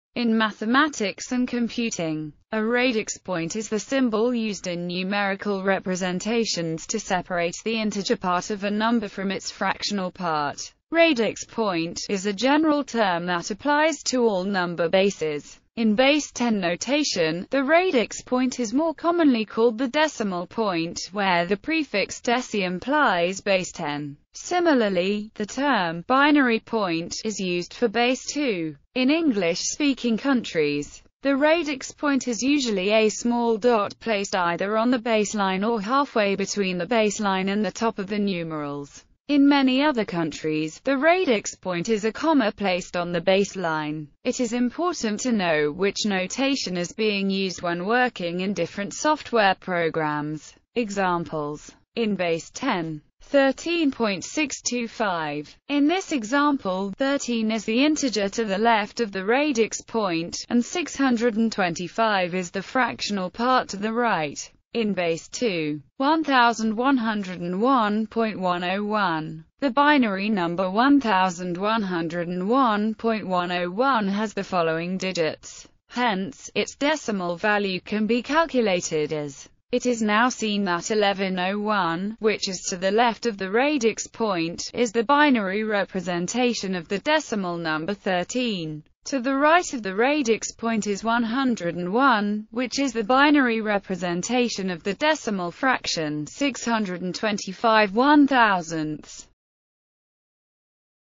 Radix point
In mathematics and computing, a radix point (or radix character) is the symbol used in numerical representations to separate the integer part of a number (to the left of the radix point) from its fractional part (to the right of the radix point)."Radix point" is a general term that applies to all number bases.